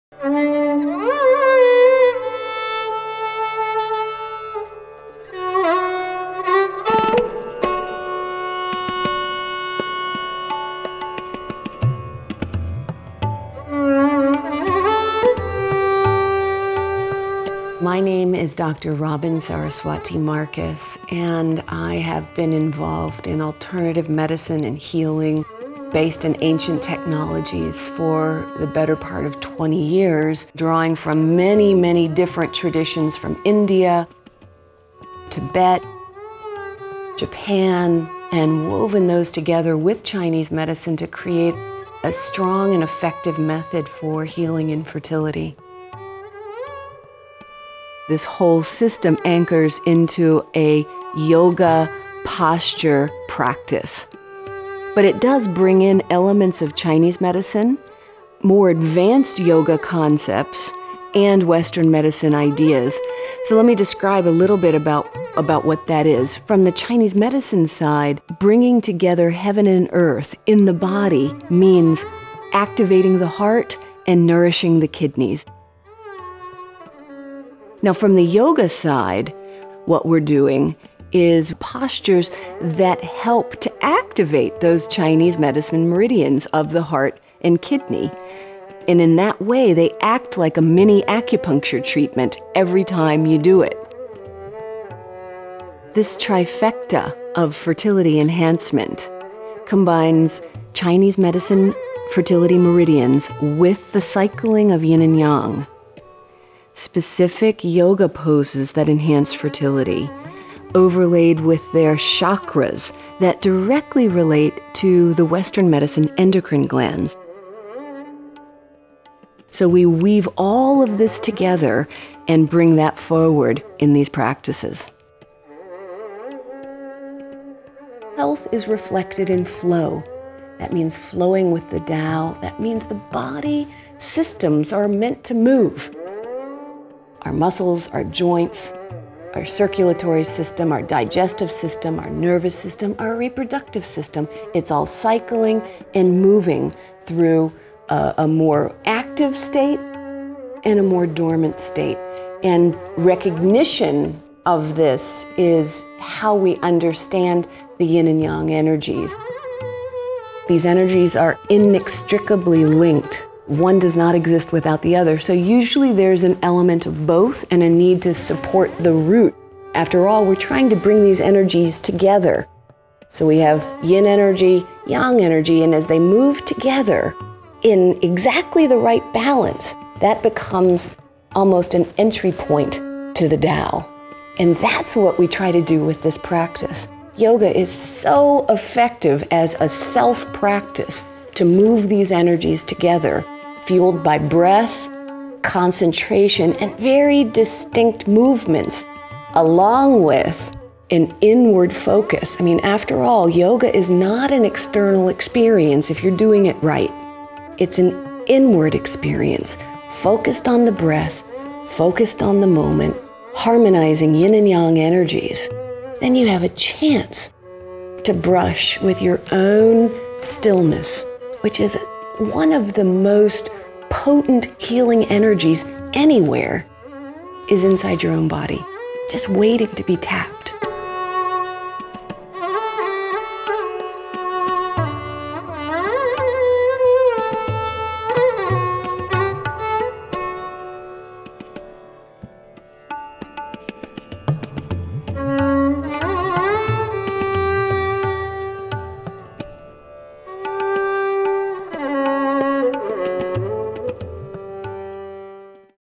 Dao Flow Yoga for Fertility & Women's Health | Trailer | Dr Saraswati | Nourishing Life
Heal Yourself At Home!
Dao Flow Yoga revitalizes your entire reproductive system to balance your hormones, reclaim and optimize fertility, or ease the transition into menopause with these powerful yoga practices.